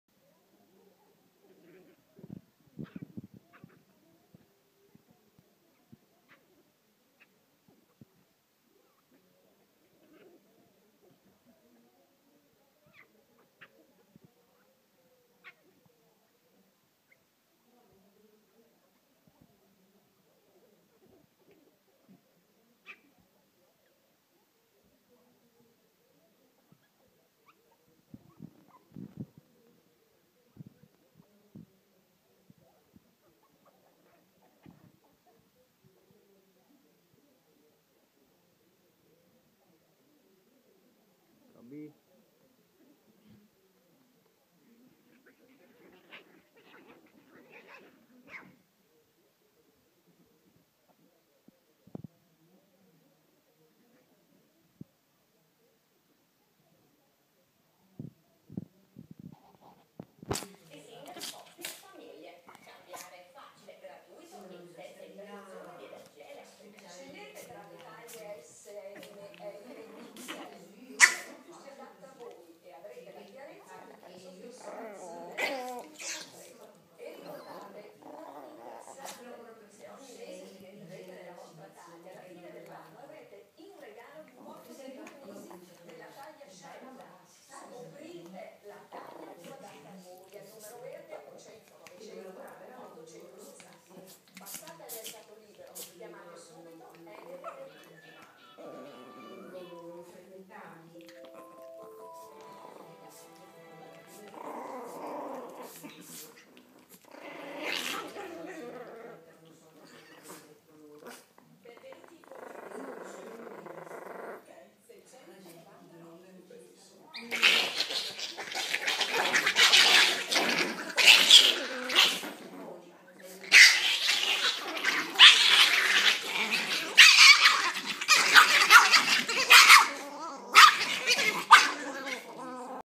Furbizia femminile
Il furto Dell osso lana la mia femmina di chihuahua sfrutta ogni espediente per ottenere quello che vuole anche i suoi cuccioli